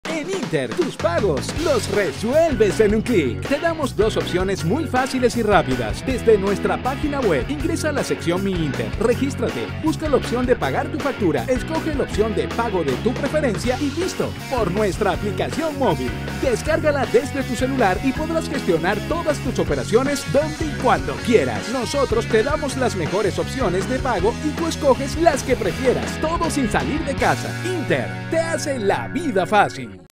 ¡Paga tu factura por mi Inter o App Móvil!
Tienes dos opciones para estar al día, a través de la herramienta Mi Inter y de la App Móvil puedes pagar tu factura.